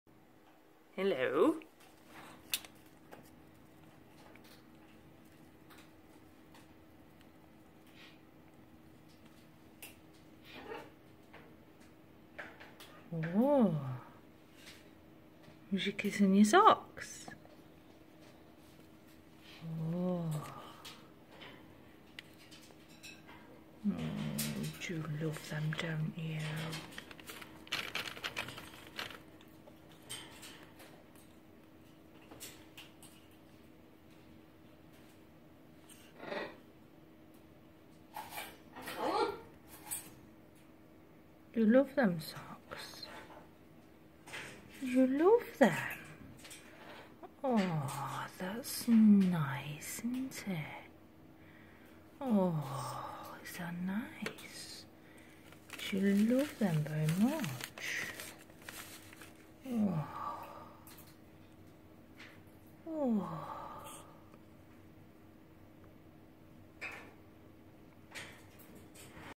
Harley loves his socks
Harley is feeling the hormones and his socks help. He lives them feeds them looks after them